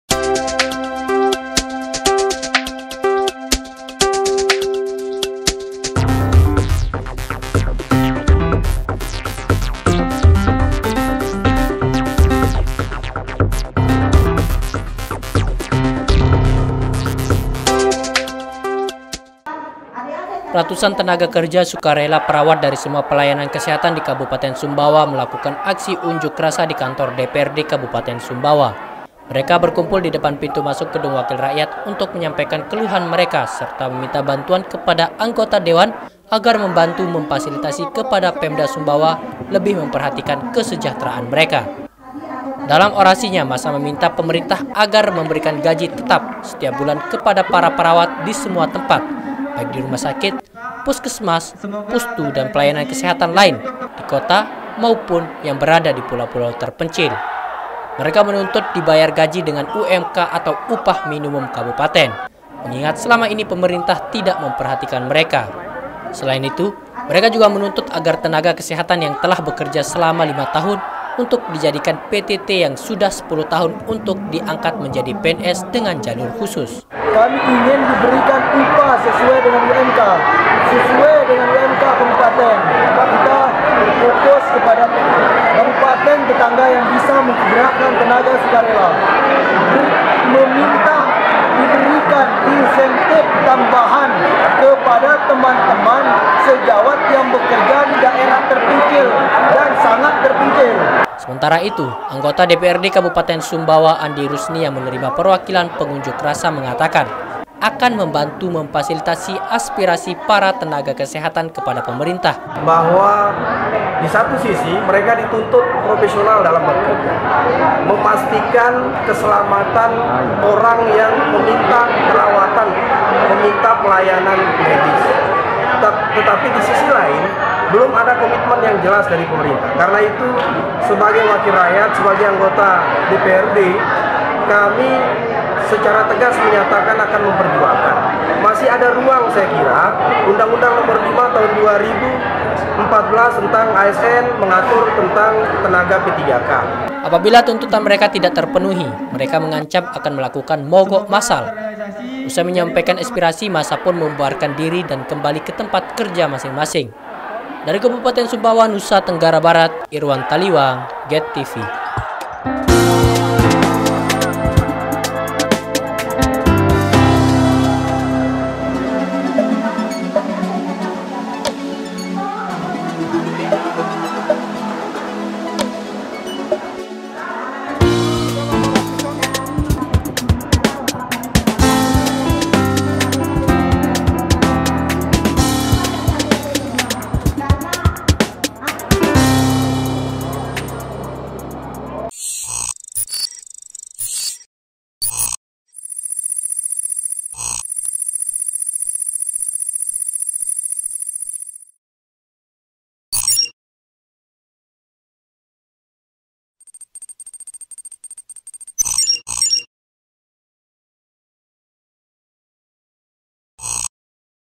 RATUSAN TENAGA KESEHATAN GEDOR DPRD SUMBAWA
Ratusan perawat dari seluruh pelayanan kesehatan di Kabupaten Sumbawa,  Nusa Tenggara Barat, melakukan aksi unjuk rasa ke gedung DPRD Sumbawa.

Para tenaga sukarela ini menuntut pemerintah memberikan gaji kepada perawat setiap bulan, sesuai upah minimum Kabupaten atau UMK.